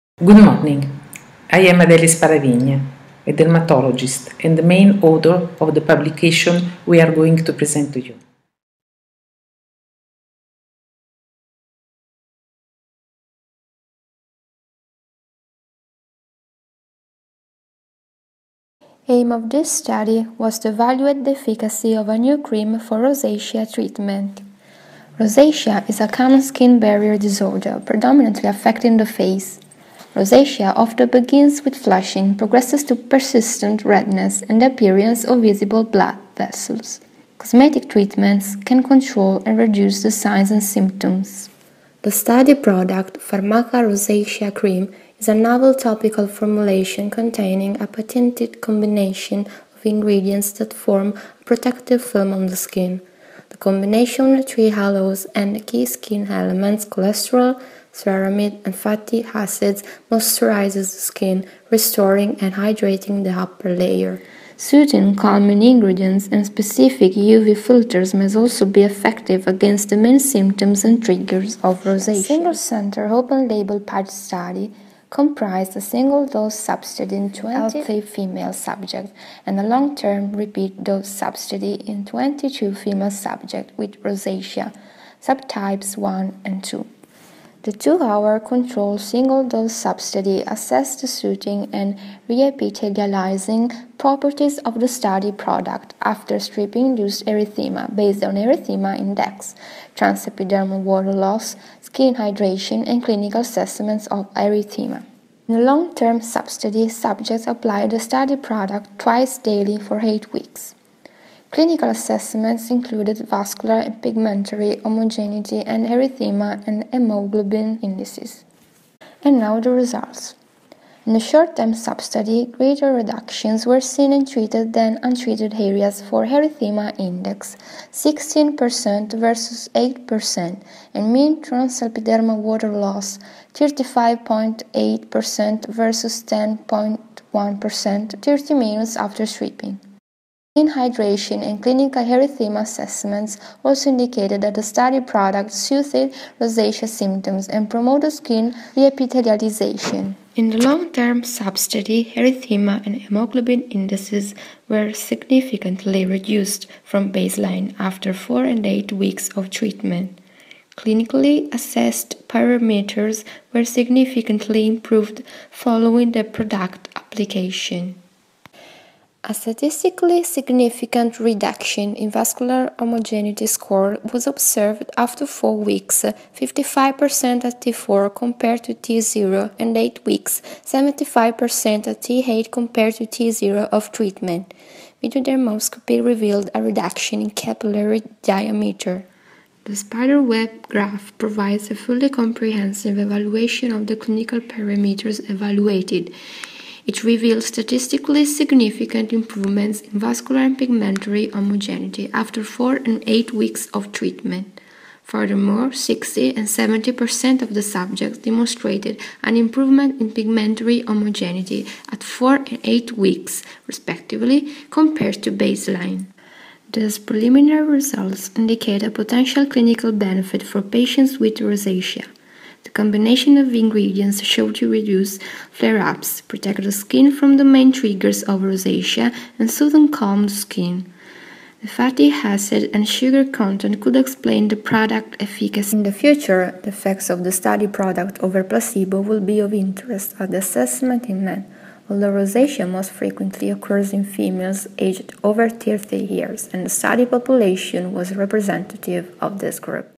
Clinical evaluation of a novel topical formulation for rosacea - Video abstract [69410]
Video abstract of original research paper Preliminary open-label clinical evaluation
of the soothing and reepithelialization properties of a novel topical formulation for rosacea published in the open access journal Clinical, Cosmetic and Investigational Dermatology by Adele Sparavigna, Beatrice Tenconi and Ileana De Ponti.

Background: Rosacea is a common, incurable skin barrier disorder characterized by relapses and remissions.

Purpose: To evaluate the efficacy of Farmaka Rosacea Cream (FRC), a novel topical formulation for rosacea.

Methods: This single‑center, open‑label pilot study comprised a single‑dose substudy in 20 healthy subjects and a long‑term, repeat‑dose substudy in 22 subjects with rosacea. The 2‑hour, controlled, single‑dose substudy assessed the soothing and reepithelialization properties of FRC after stripping-induced erythema based on the erythema index, transepidermal water loss, skin hydration, and clinical assessments of erythema. In the long‑term substudy, subjects applied FRC twice daily for 8 weeks. Clinical assessments included vascular and pigmentary homogeneity and erythema and hemoglobin indices. Subjects completed questionnaires to assess FRC efficacy and cosmetic acceptability.

Results: Greater reductions were seen in FRC‑treated areas compared with untreated areas for the erythema index (-16% versus -8%; P,0.001) and mean transepidermal water loss (-35.8% versus -10.1%; P,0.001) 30 minutes after stripping. Significant improvements over untreated areas were maintained 2 hours after stripping. Skin hydration and clinical erythema assessments also indicated that FRC soothed rosacea symptoms and promoted skin reepithelialization. Erythema and hemoglobin indices were significantly reduced from baseline after 4 and 8 weeks of treatment. Clinically assessed parameters were significantly improved following FRC application. Subjects assessed FRC positively.

Conclusion: Improvement of rosacea symptoms was noted with FRC application. The main film‑forming ingredients of FRC (trehalose, cholesterol, ceramide, and fatty acids), combined with other soothing and calming ingredients and ultraviolet filters, could explain its efficacy.